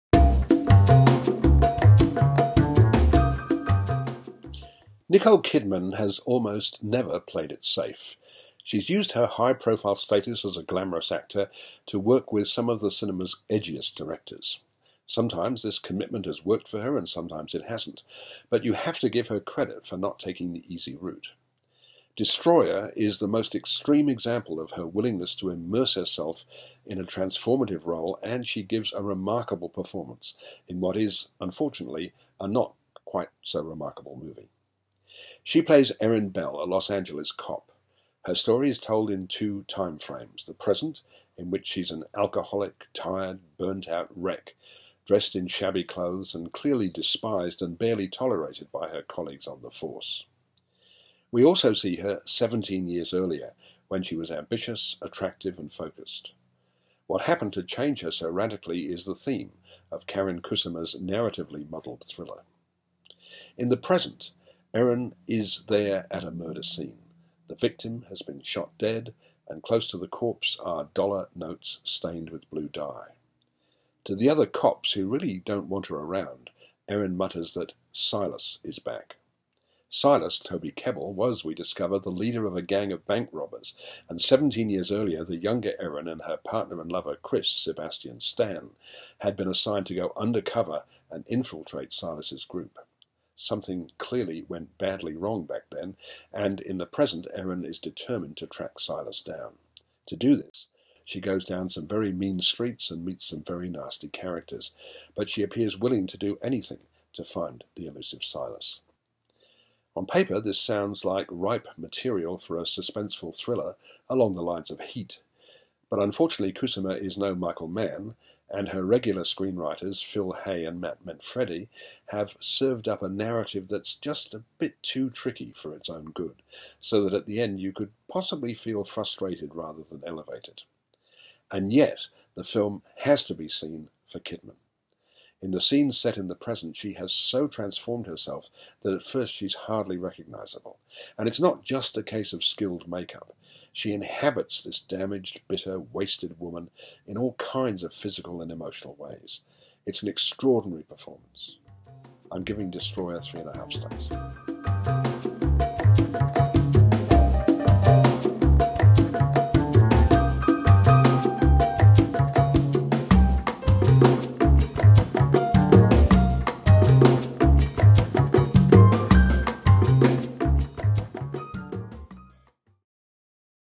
David Stratton Reviews Destroyer
David Stratton Reviews Destroyer at Palace Nova

LAPD detective Erin bell must face the ghosts of her past when a gang leader resurfaces, forcing her to reconnect with people from her rookie days.